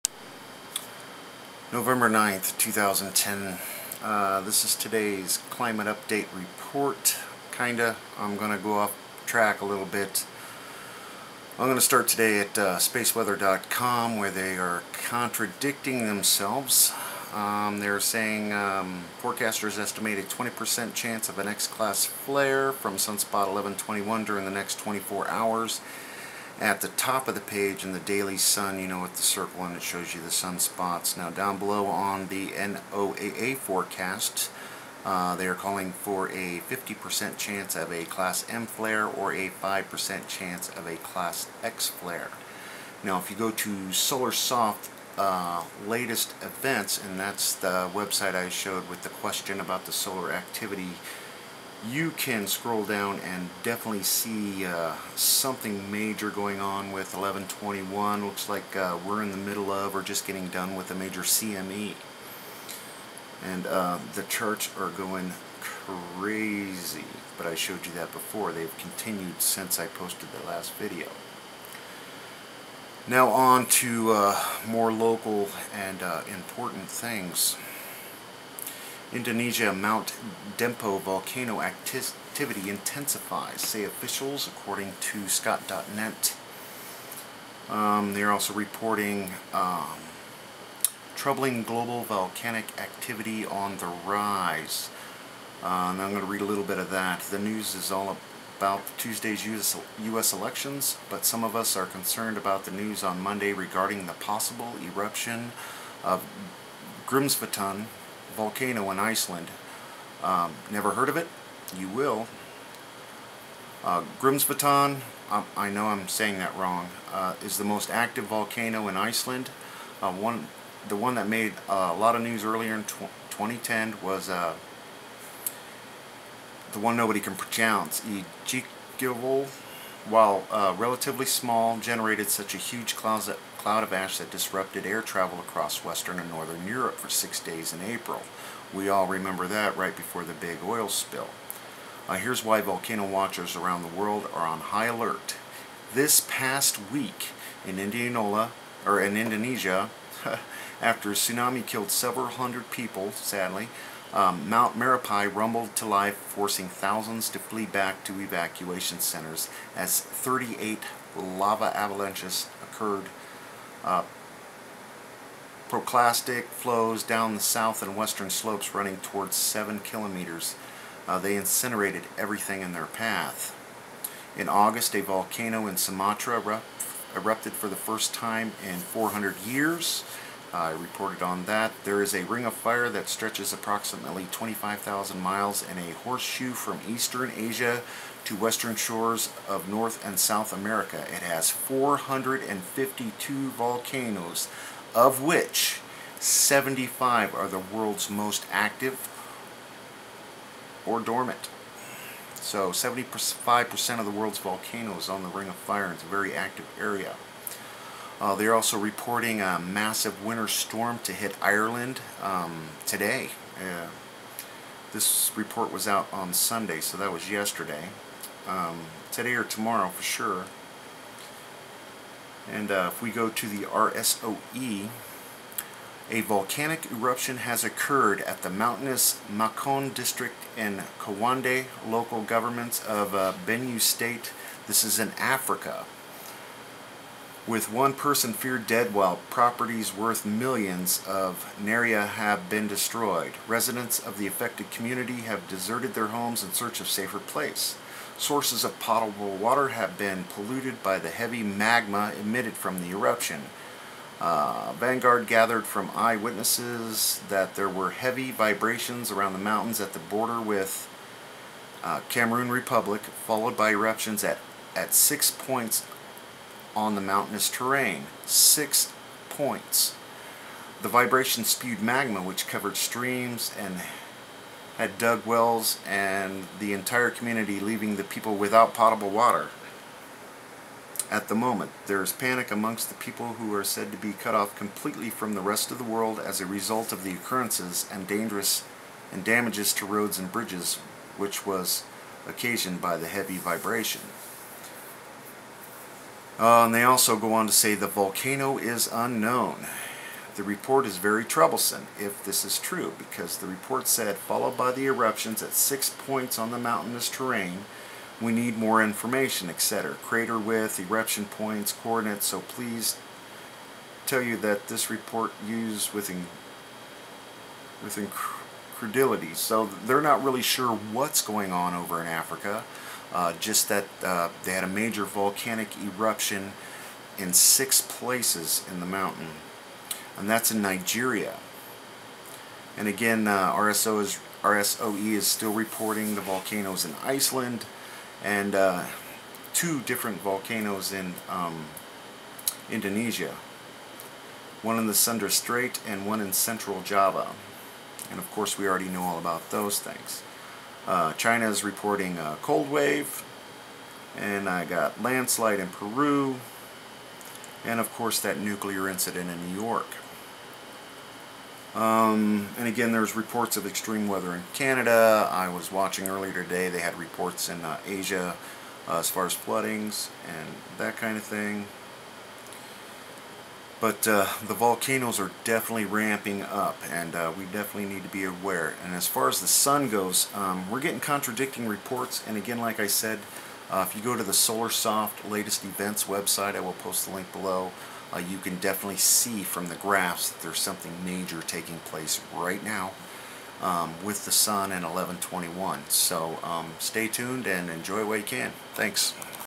Volcano Eruption Africa (09 November 2010) Climate Change Update
Big things going on with the Sun... Volcano erupts in Nigeria..Massive damage

Spaceweather giving two sets of numbers and SolarSoft is showing a lot of activity.